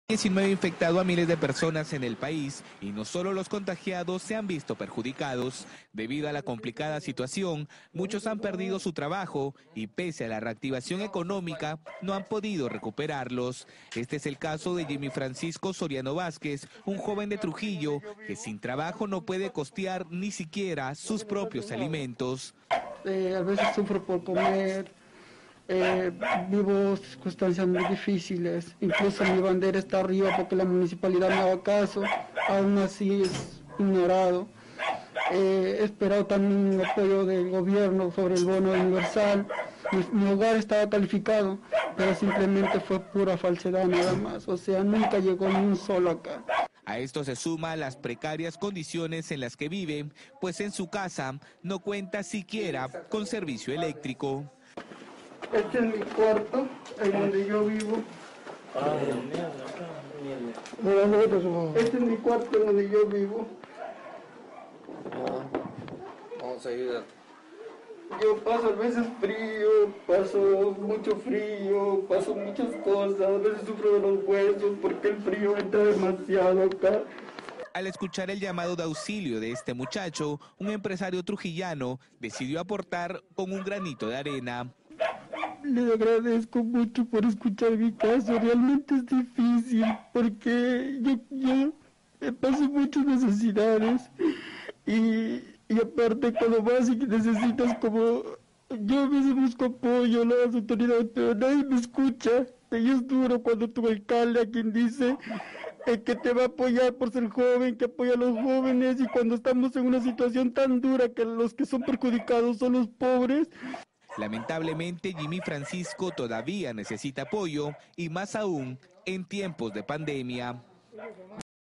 Cadena de solidaridad: ayudan a joven que perdió empleo por pandemia
Muchos han perdido su trabajo debido a la complicada situación que nos tocó vivir a causa del coronavirus. Pese a la reactivación económica, cientos de ciudadanos no han podido recuperarse económicamente.